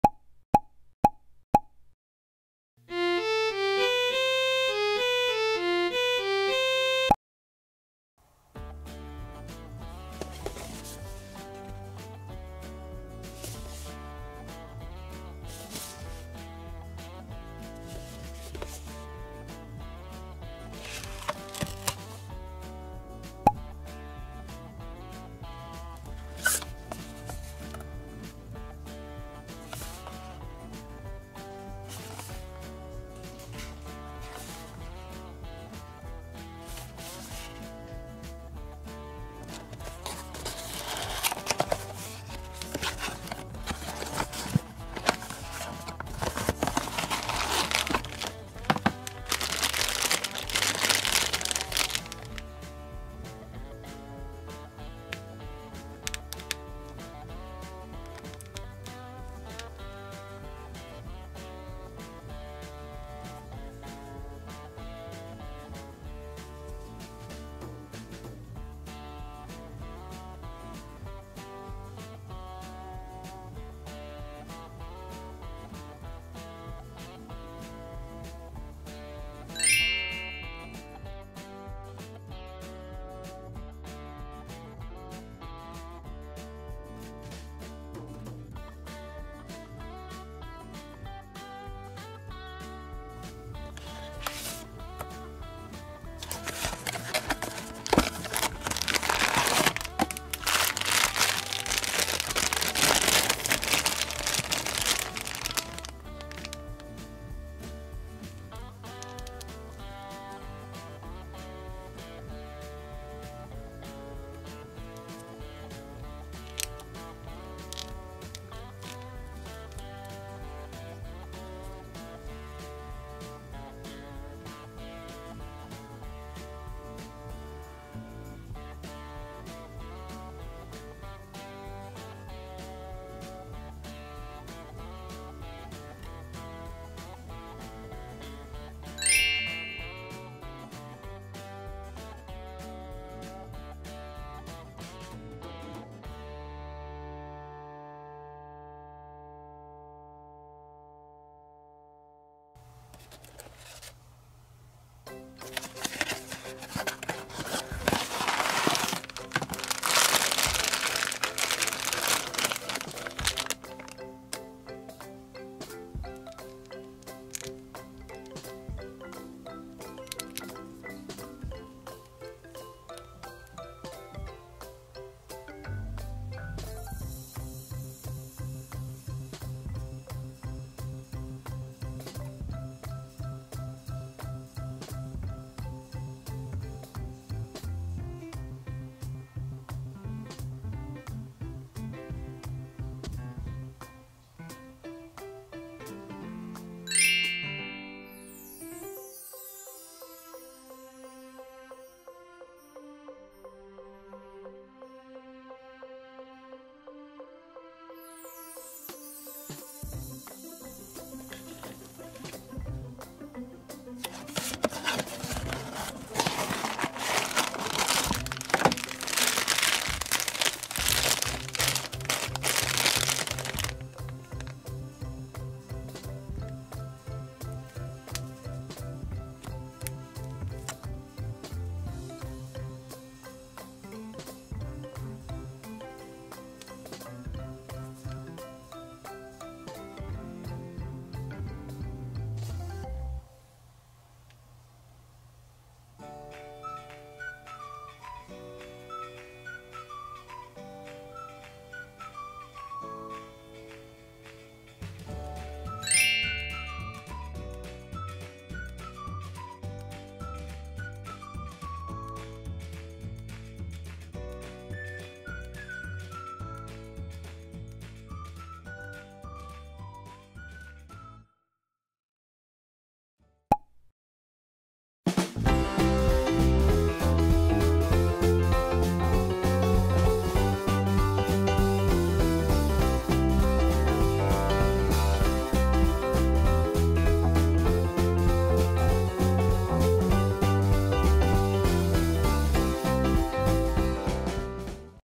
プチラマ ドラカプリバース 超絶限界突破編『1BOX 開封』ドラゴンボール Dragonball Diorama Figure
↓プチラマ ドラカプリバース 超絶限界突破編↓
※上記URLはAmazonアソシエイトのリンクを使用しています。

種類は全部で４種
１個９００円（税抜き）

・孫悟空＆孫悟天
・10倍界王拳
・ベジータ
・孫悟空vsゴールデンフリーザ